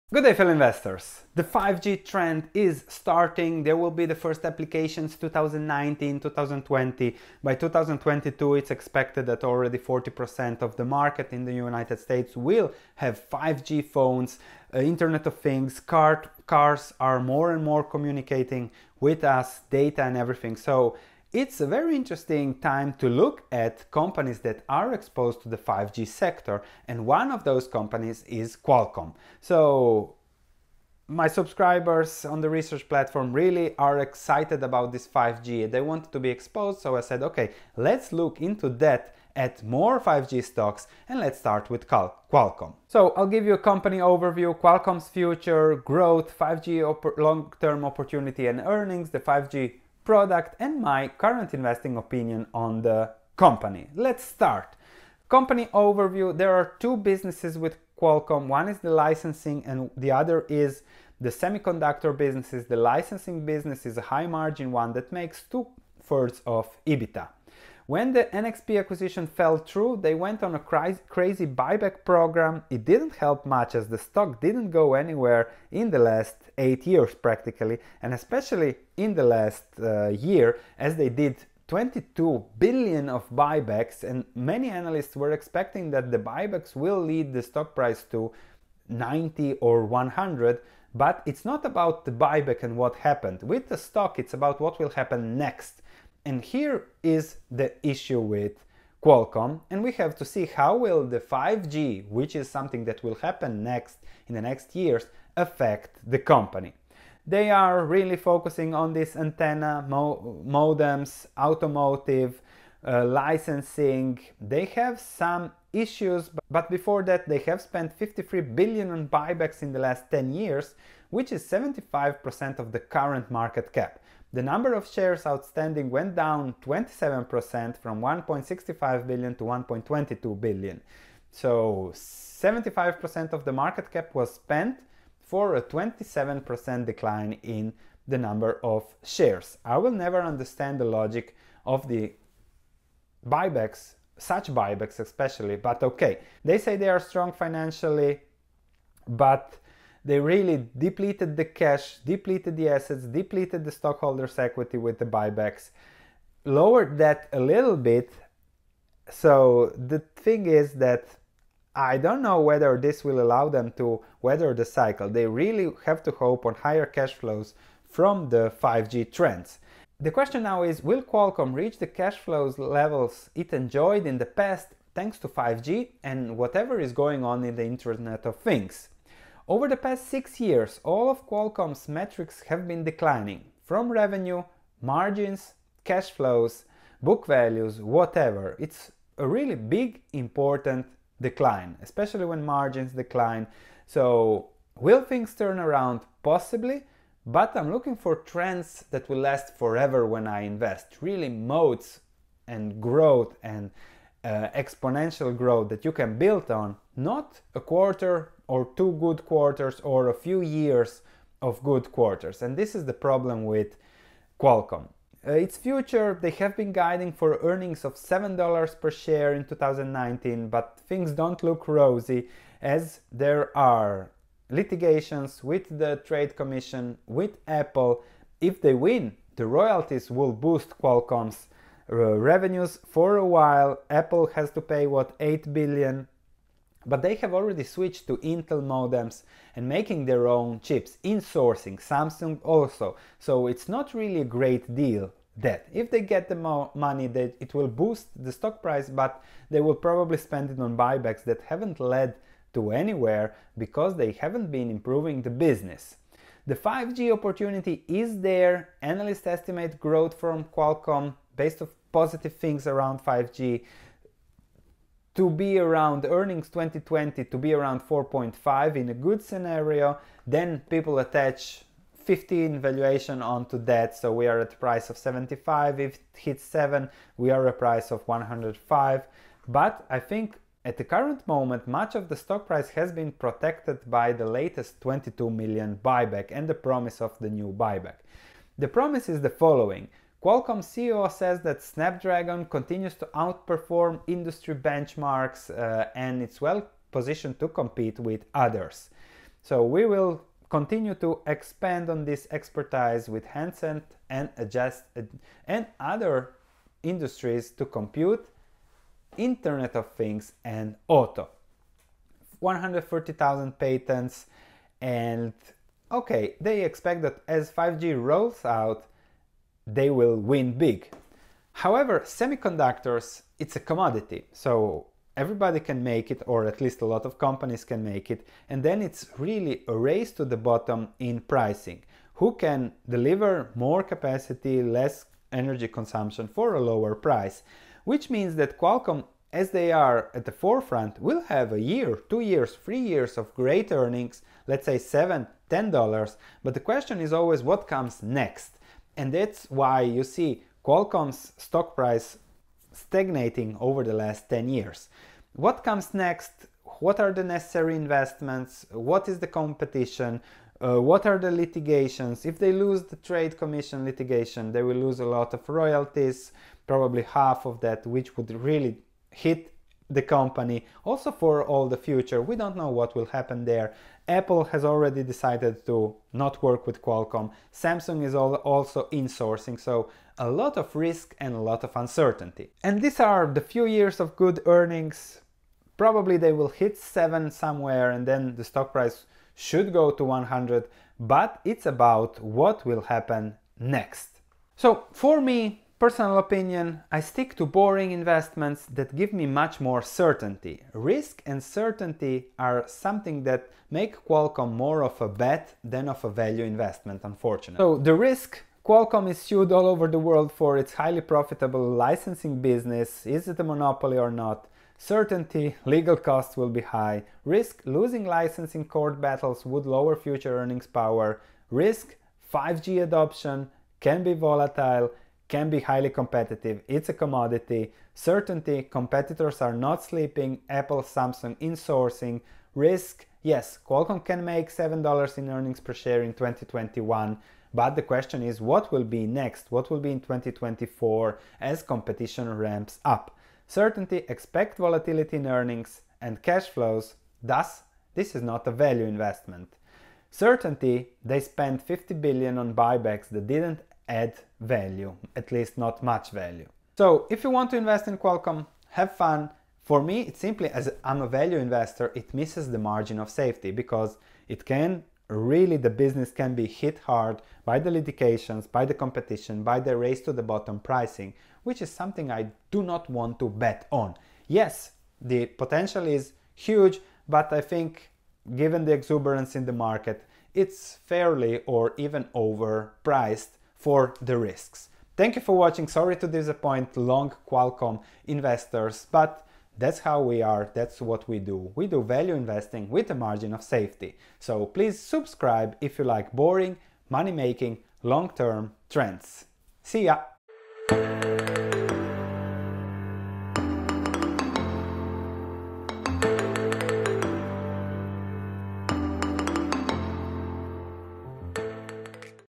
Qualcomm QCOM Stock Analysis - 5G Investing Opportunity
5G is coming and many with to have their stock market portfolios exposed to the trend. I analyse QCOM in my attempt to find interesting 5G investments with a margin of safety.

I am also a book author:
Modern Value Investing book: